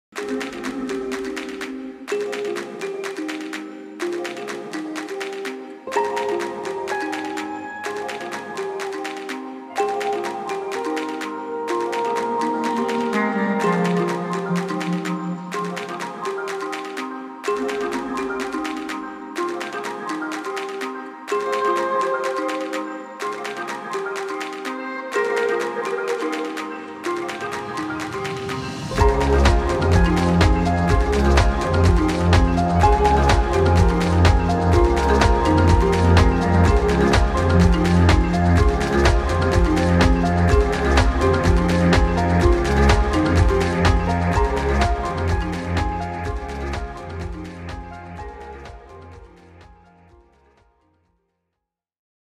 Wisteria Avenue: The Making of our Spring Blooms
Watch our spring blooms come to life: made with over 500 metres of paper and 15 litres of ink.